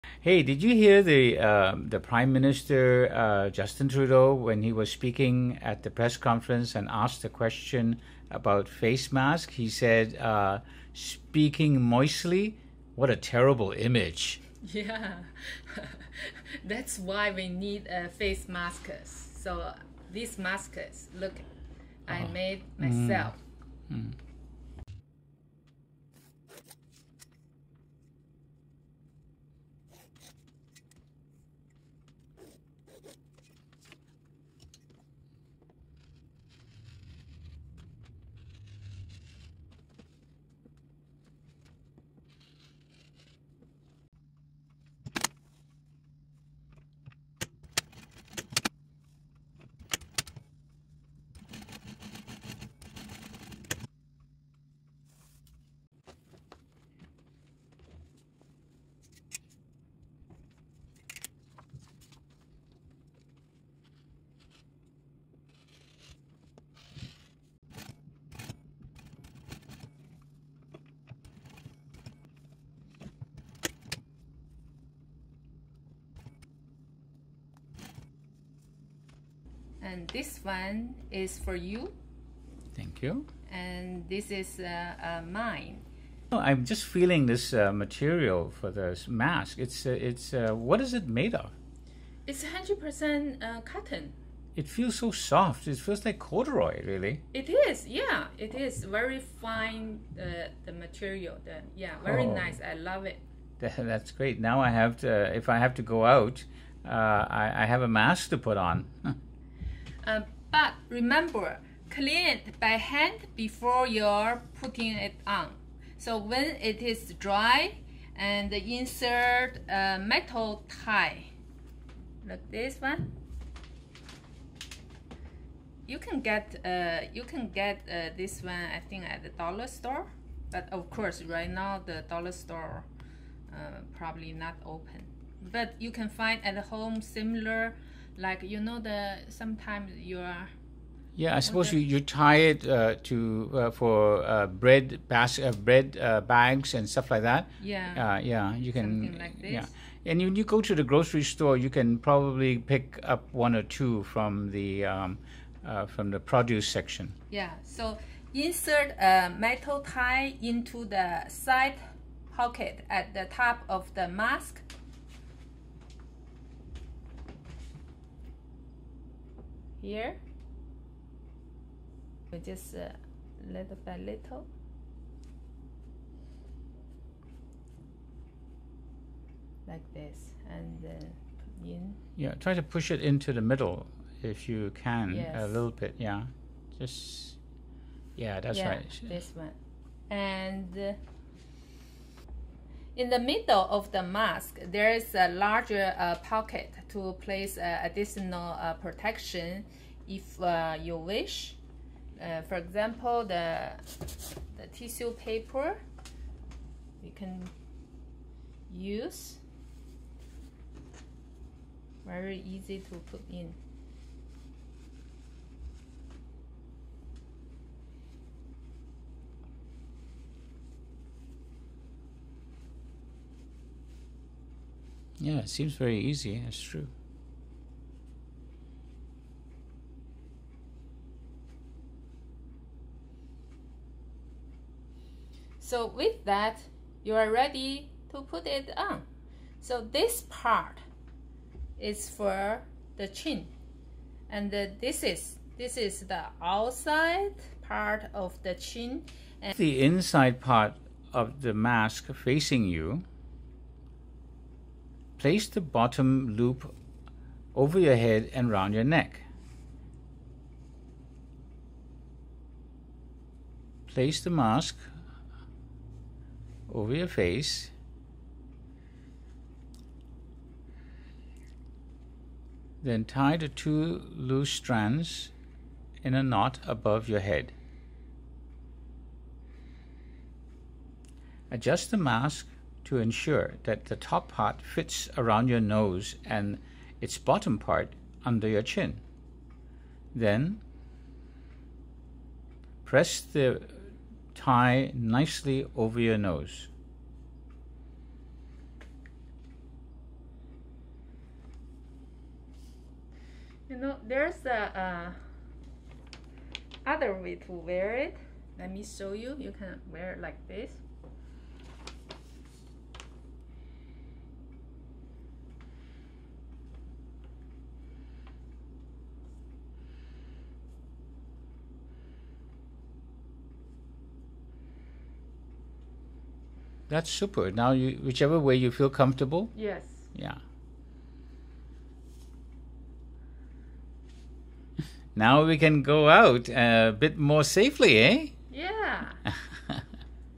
"Speaking Moistly" Mask
The Prime Minister of Canada, Justin Trudeau, said, "Speaking mostly. what a terrible image."
That's why we need masks when we have to go out, that may help our communities stop the pandemic.